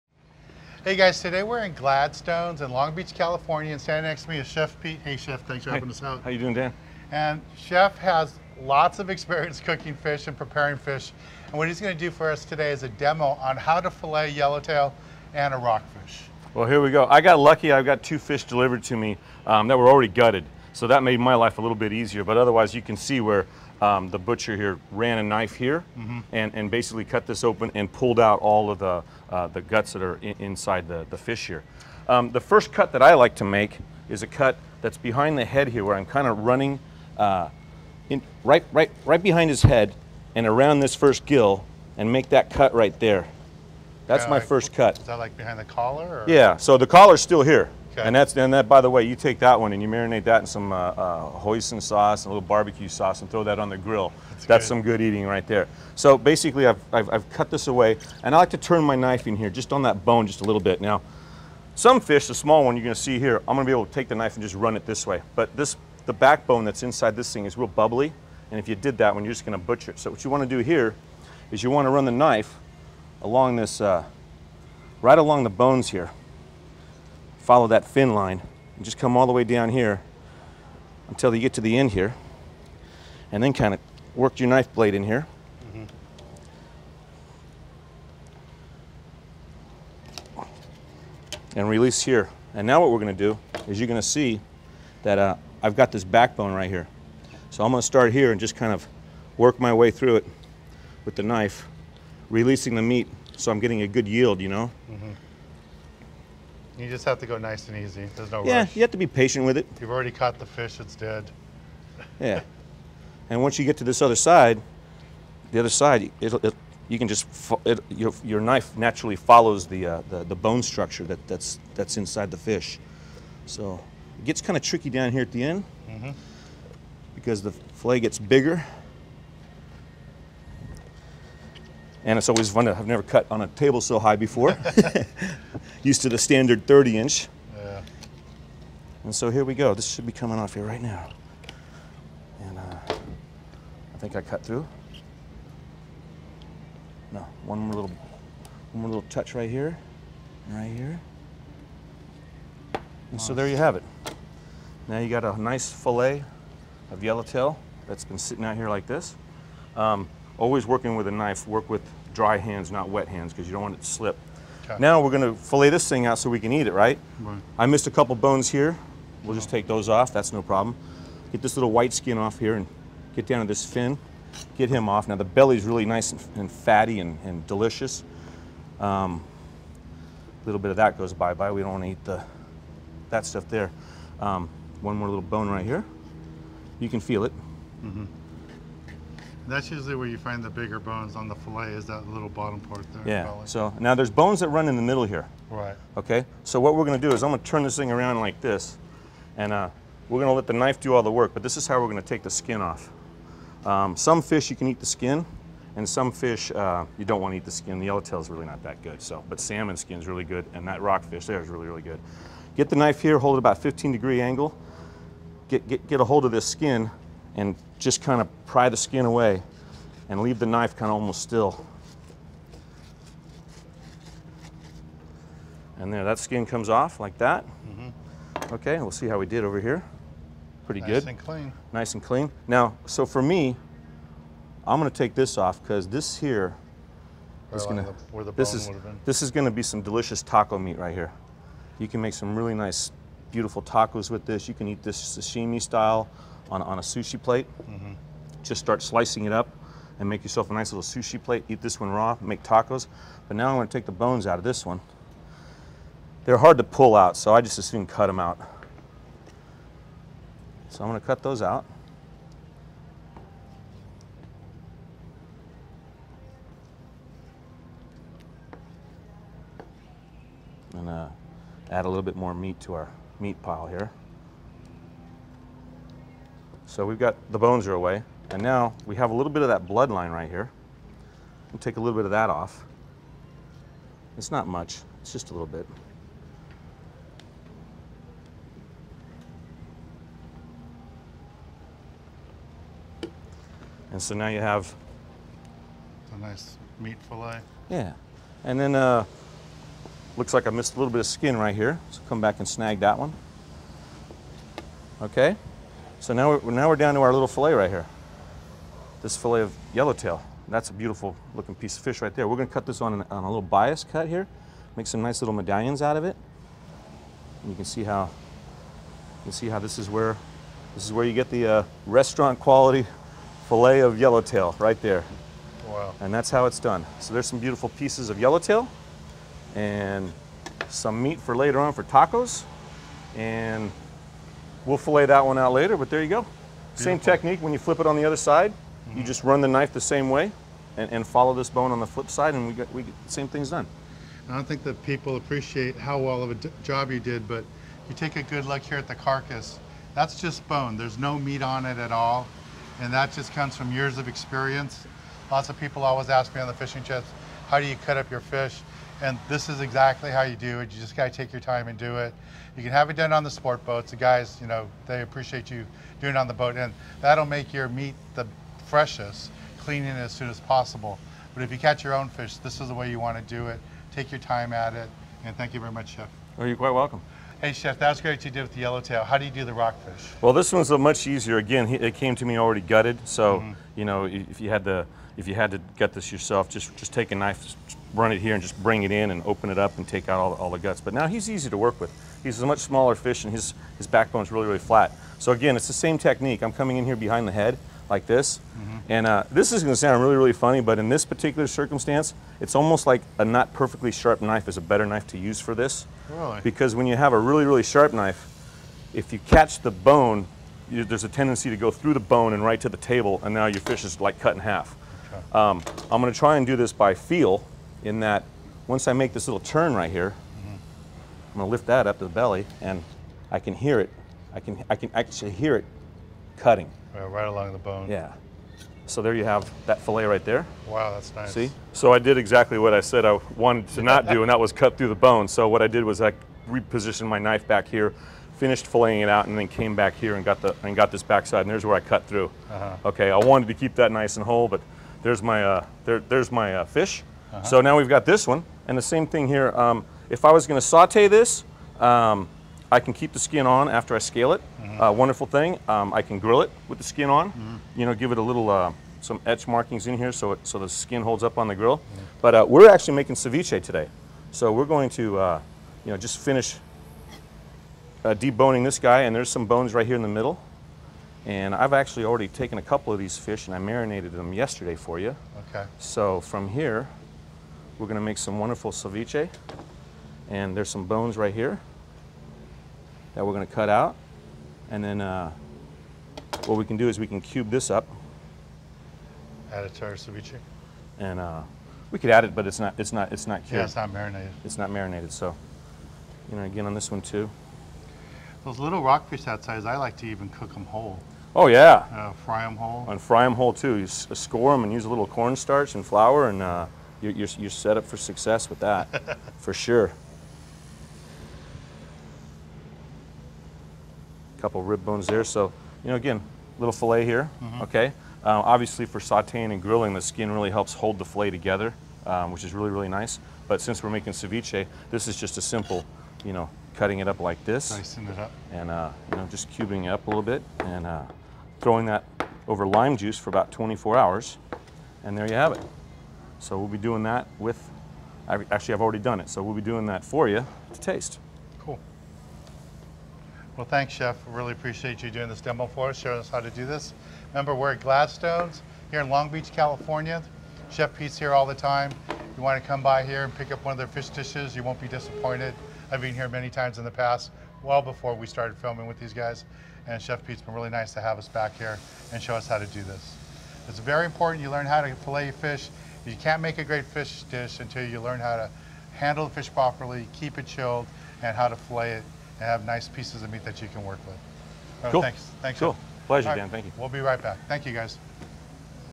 How -To Fillet A Yellowtail & Rock Fish | SPORT FISHING
Chef Pete from Gladstones in Long Beach, CA shows us the proper way to fillet a yellowtail & a rock fish.